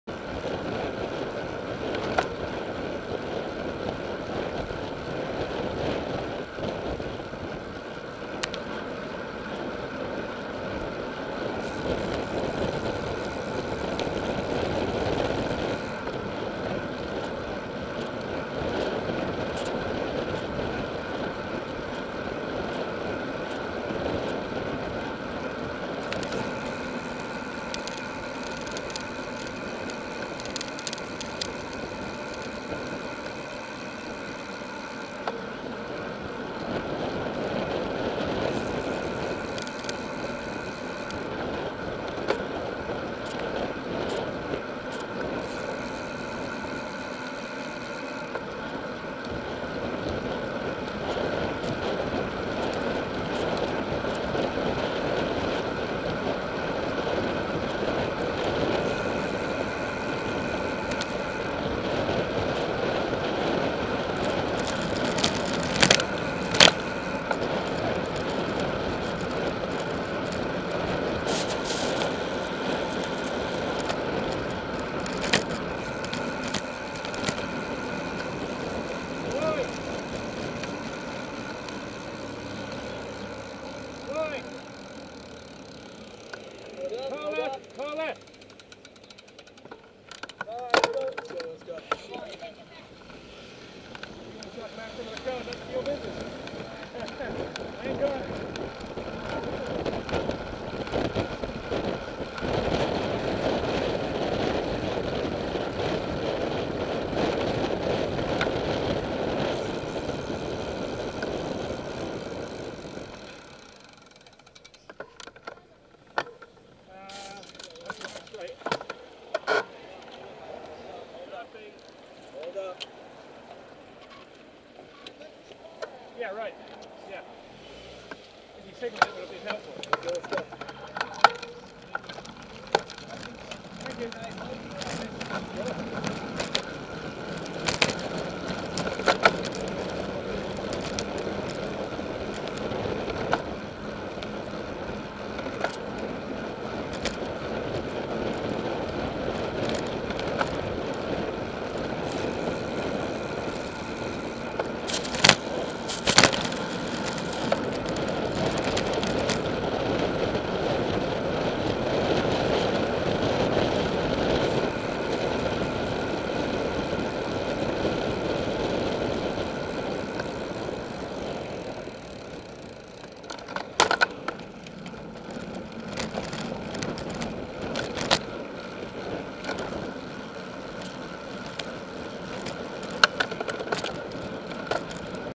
04_Sat: Cyclists Are Radioactive...
...so I have to drive on the wrong side of the road (BikeMS Coast the Coast 2012).